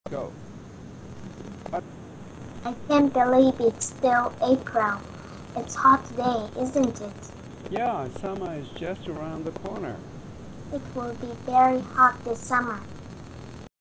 Roll playing with Megu: lesson 14
Shelly: I can’t believe it’s still April. It’s hot today, isn’t it?
Laura : Yeah, summer is just around the corner.
Shelly: It will be very hot this summer.